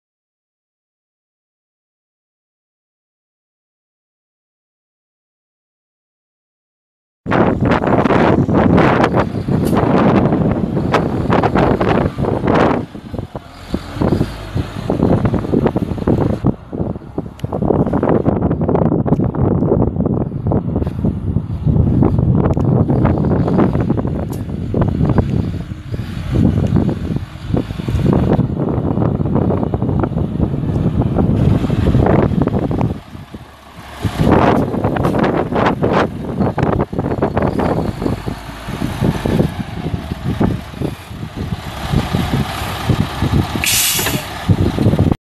2005 Sterling L8500 semi truck for sale | sold at auction February 18, 2016
2005 Sterling L8500 semi truck
331,805 miles on odometer
Single axle
Pintle hitch
Dual fuel tanks
Quarter fenders
Air suspension
Air brakes
11R24.5 tires
2FWBAVDC15AV45305